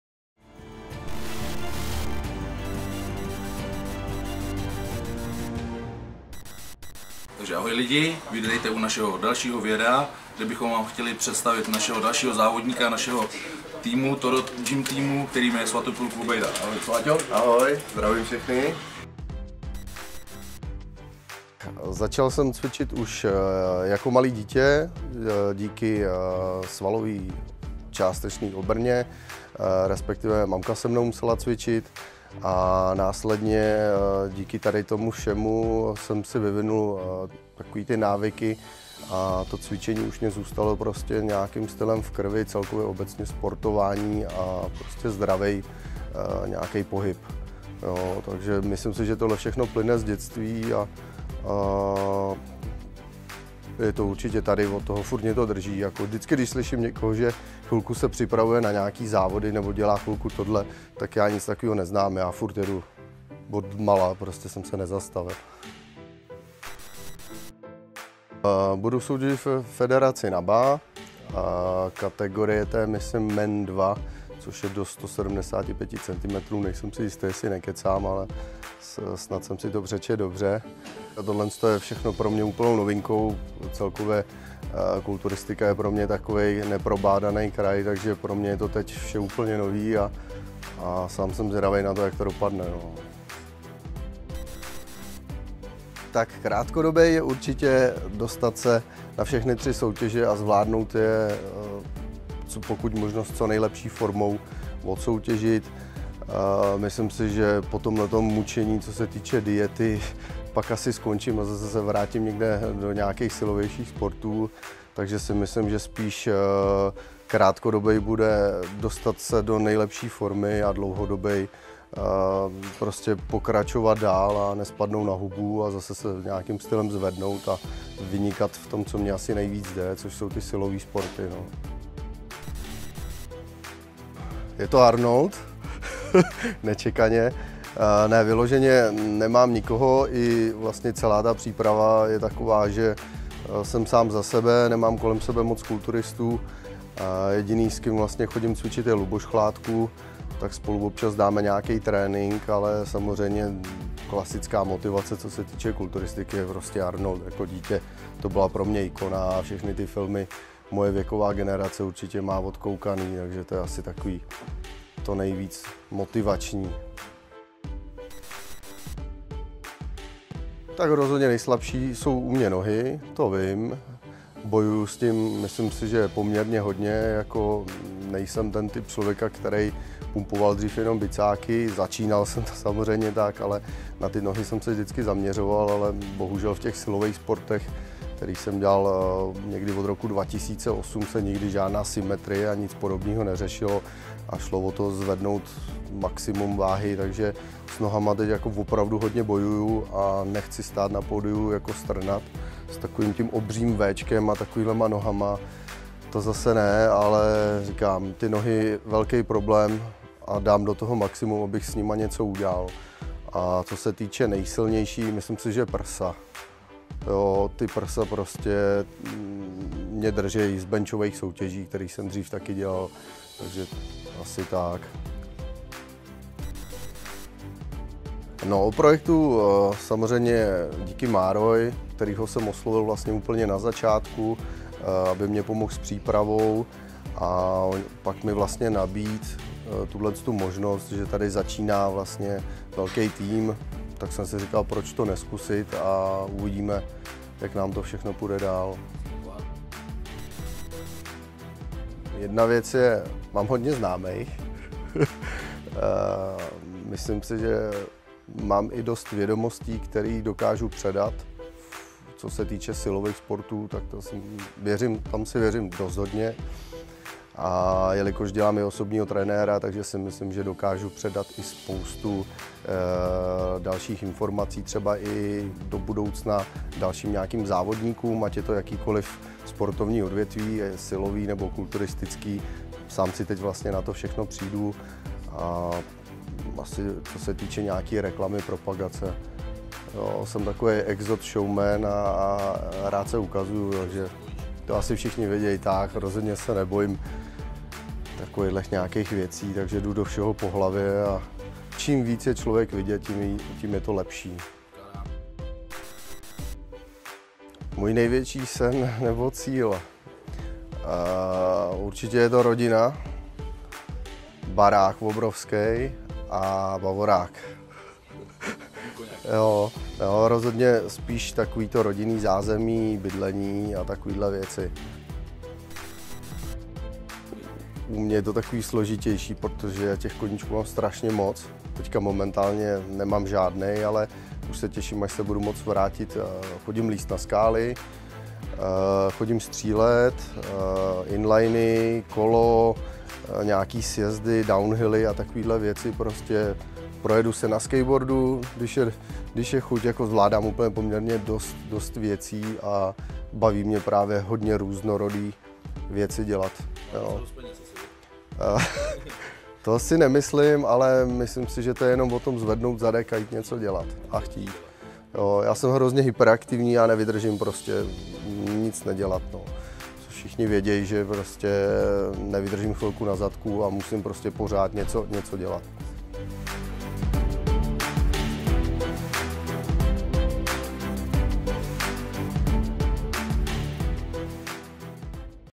Svatopluk Vobejda - ToroGym Team
Představení závodníka Svatopluka Vobejdy v kategorii Kulturistika z ToroGym Teamu.